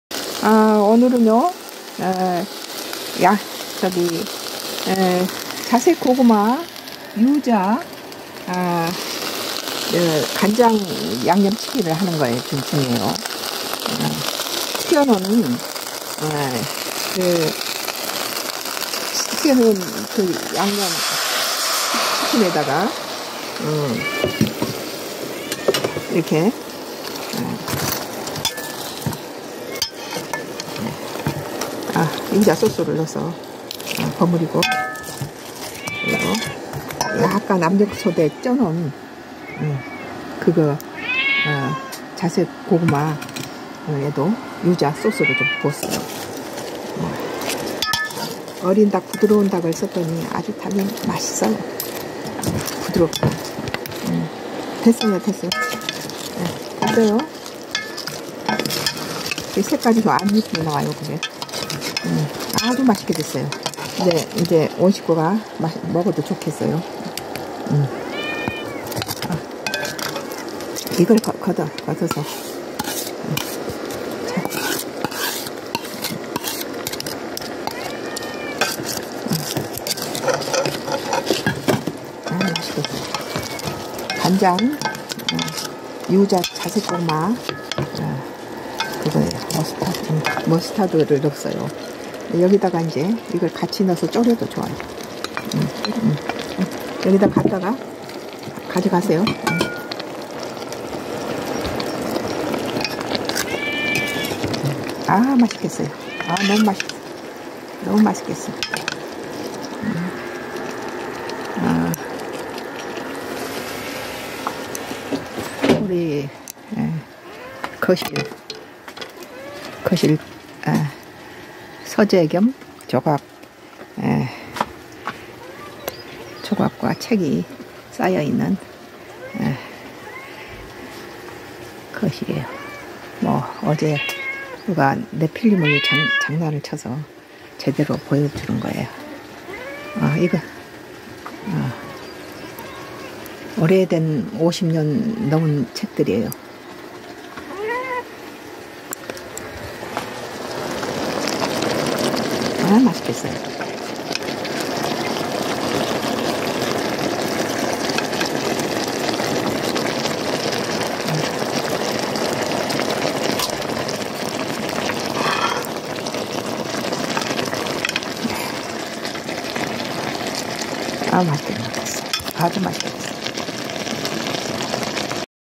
MAMA FOOD Mom’s ticket! Yuzu, purple sweet potato soy sauce skin~~!
Mom's vote! Yuzu, purple sweet potato soy sauce skin~~!

Made with soft antibiotic-free chicken, seasoned with soy sauce, ginger, garlic, and pepper, mixed with an egg, coated with potato starch, and fried!

With soy sauce seasoned chicken sauce~~~!
Add soy sauce, garlic, ginger, sesame oil, mustard sauce, sugar, and pepper.

Add the fried chicken and the purple sweet potato that was briefly cooked in a pressure cooker to the pot.
Pour in the sauce and heat it~~~!

The whole family~~! Mustard soy sauce seasoned chicken full of yuzu flavor~~!
It was the best seasoned chicken I've ever had!